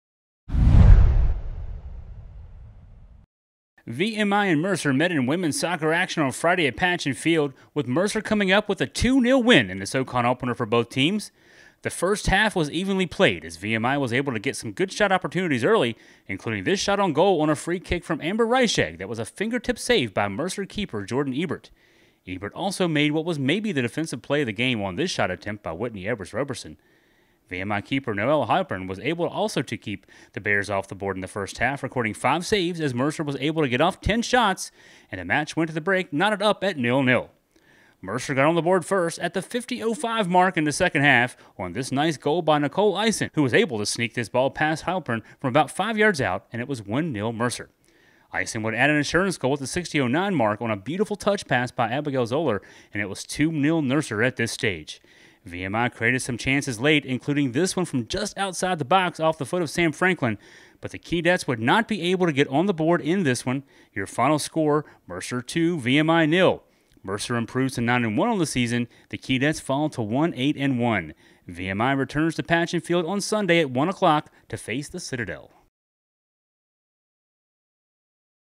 Women's Soccer vs Mercer Recap 9-27-19
VMI Women's Soccer opened SoCon play on Friday against Mercer. It was an even game for much of the match as both team traded chances. However, Mercer would strike twice in the second half to take the 2-0 win. Noelle Heilpern was solid in goal again for VMI making seven saves